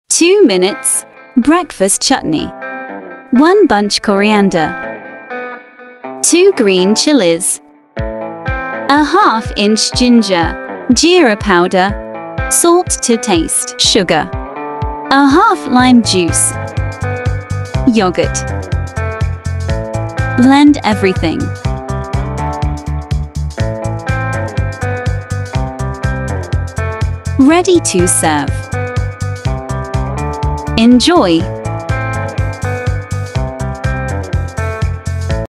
2 minutes breakfast chutney - a must try recipe!
This breakfast chutney is super easy to make. All you need is a few ingredients which you will always have in your kitchen!